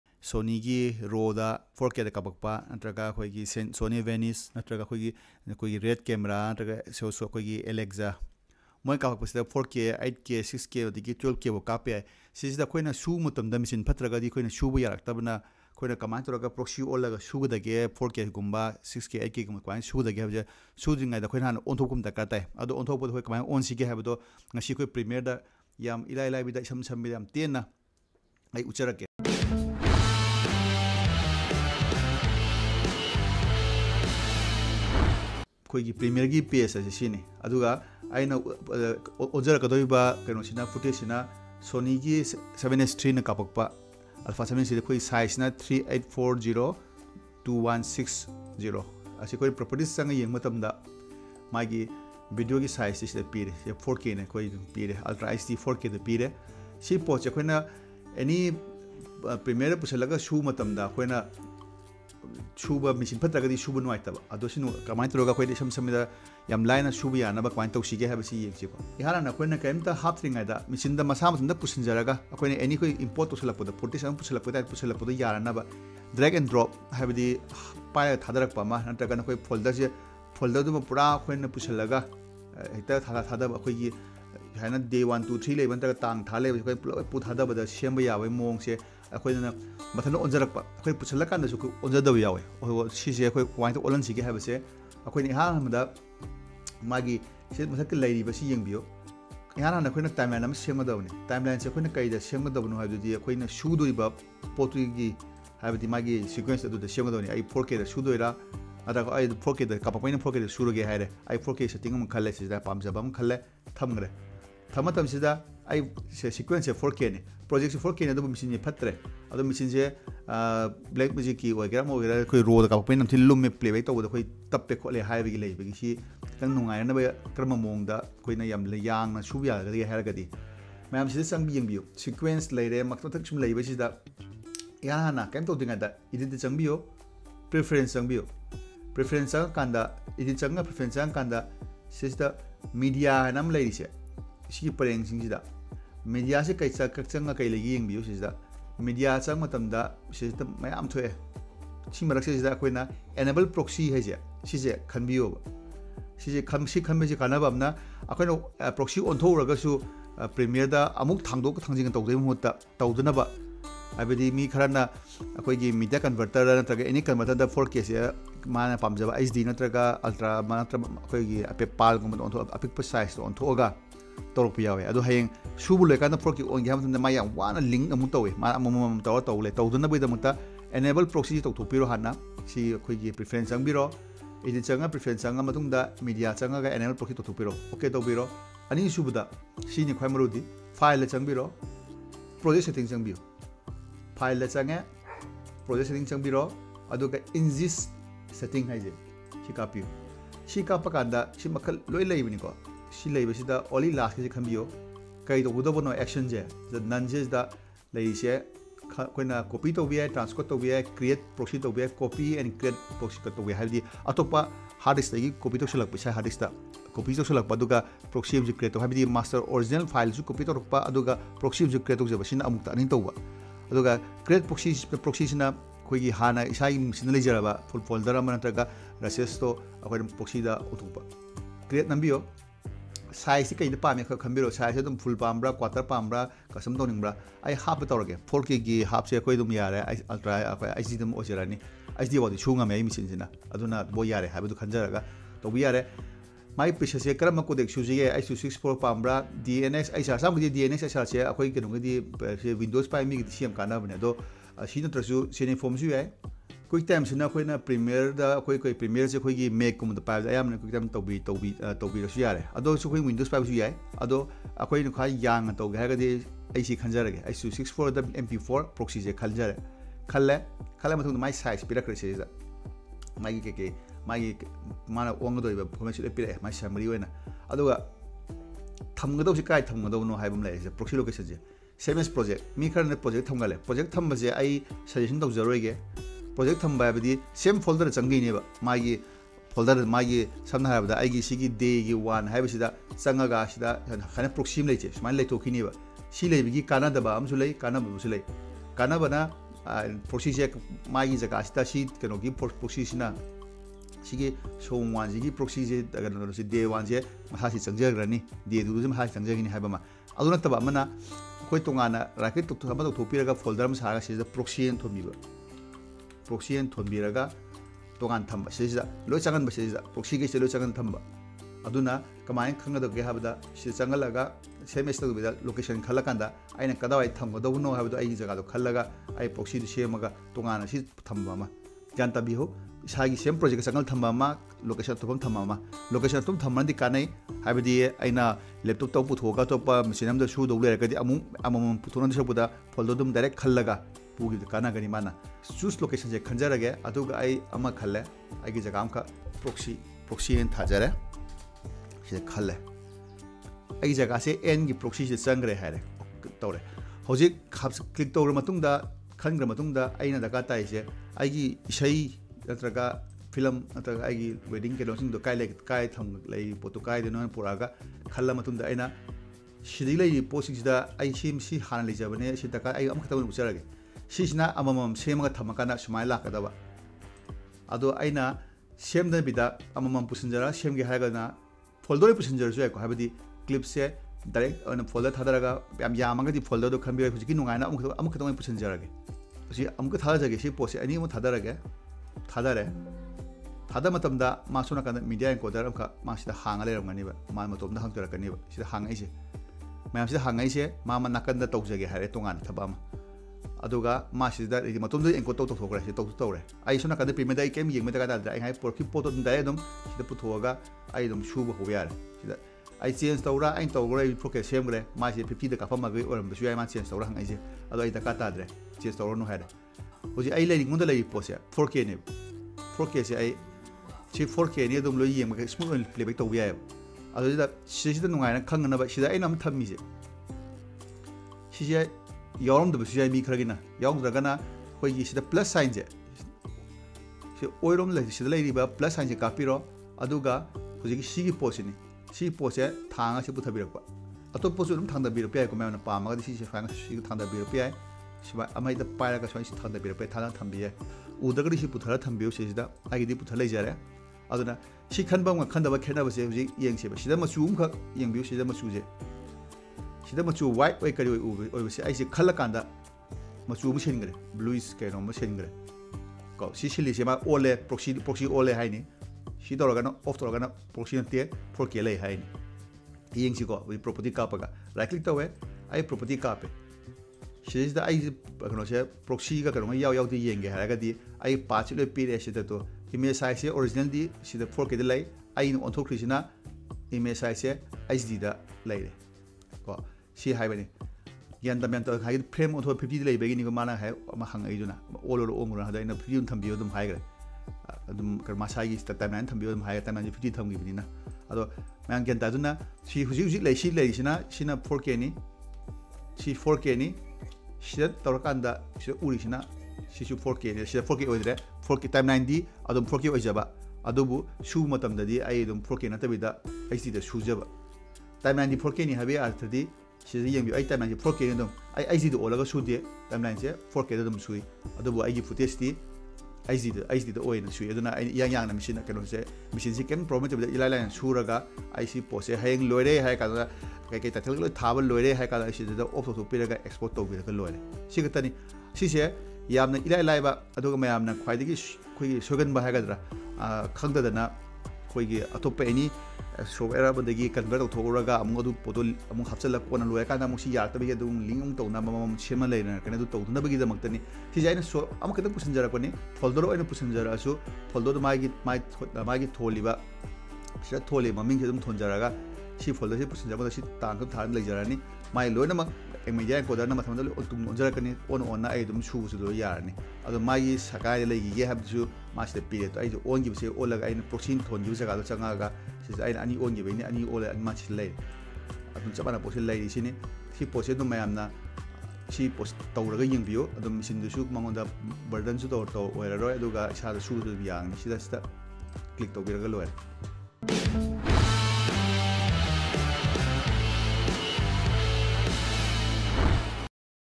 How to Create Proxies in Premiere Pro | In Manipuri
In this video, you will know How to Create Proxies video in Adobe premiere pro.

# How to create proxies video in adobe premiere.
# How to create proxy.
# 4k to HD

After this video you will know how to Create video in low resolution for east editing.

Do you have any questions, tips, or ideas about Film & Video Editing? Let us know in the
comments section below.

Footage Copyright:     @YMongsaba  @Flyhigh films
Software: @Adobe
Music  @youtubecreators